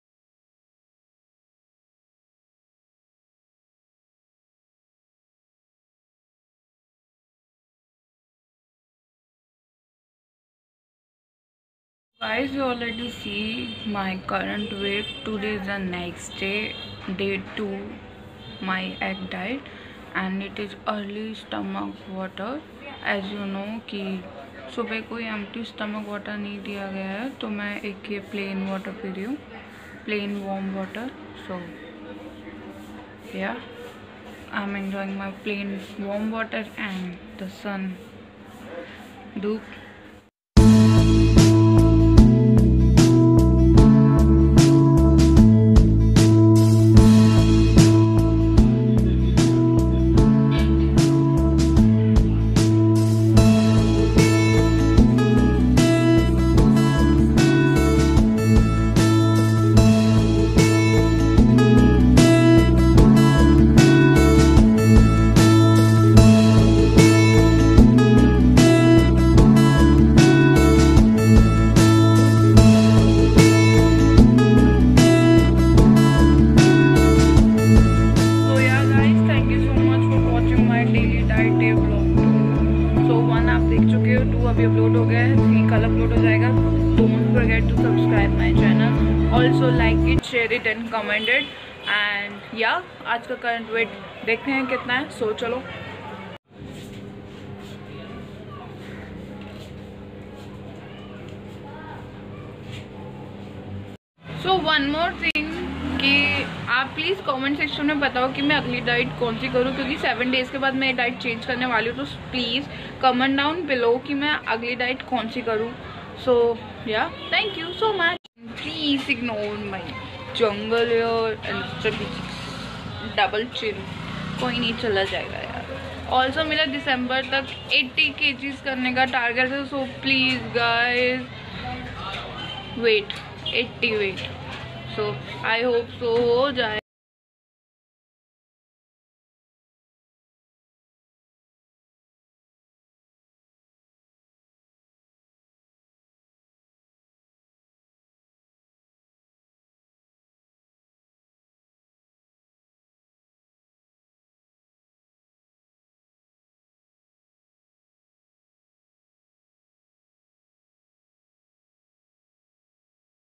TRYING EGG DIET PLAN BY VERSATILE VICKY- DAY 2| 900 CALORIES DIET PLAN
Today is the day 2 egg diet plan by versatile vicky#eggdietbyversatilevicky#versatilevicky#eggdiet#10kgsin10days

current weight also uploaded on daily basis